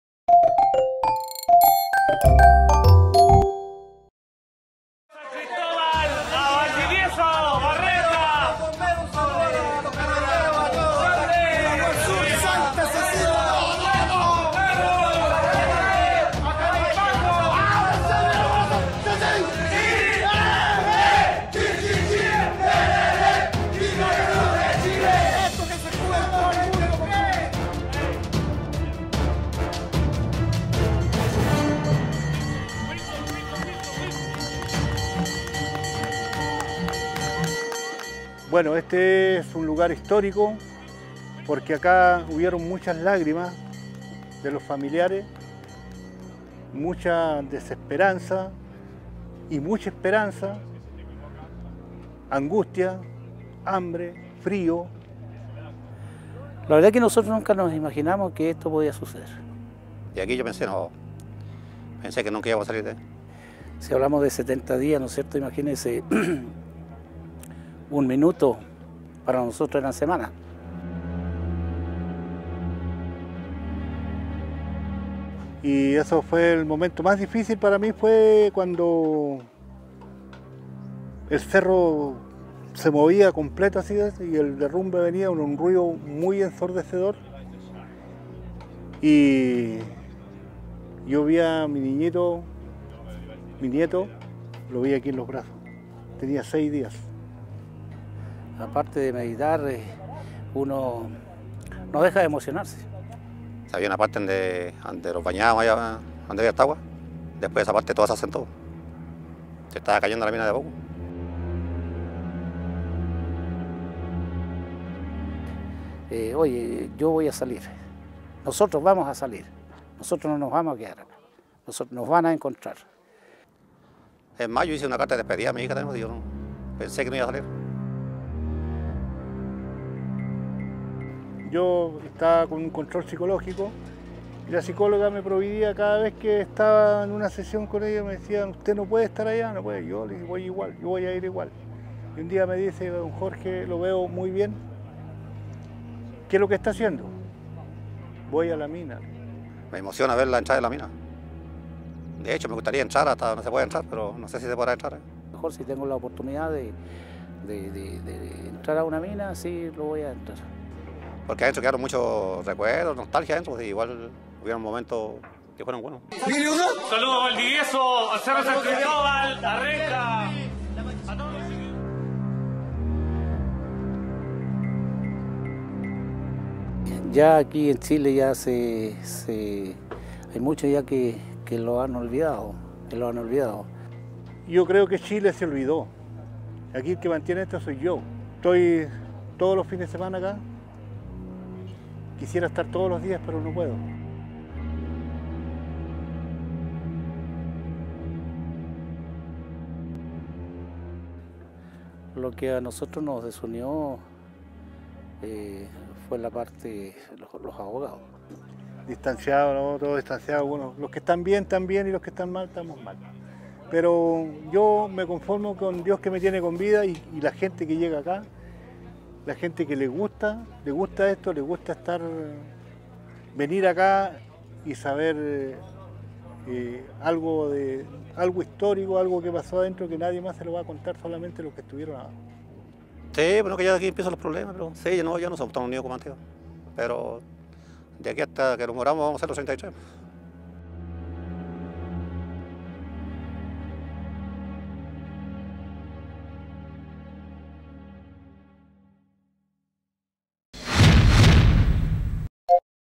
Los 33: Los héroes olvidados y separados
A seis años del llamado "milagroso" rescate de la mina San José, un grupo de los otrora héroes, buscan salvar la permanencia del pique en la tercera región como un hito histórico. Pese a la fama, reportajes y películas realizados por el evento, reconocen sentirse olvidados por los chilenos y aceptan que su separación es casi irreversible.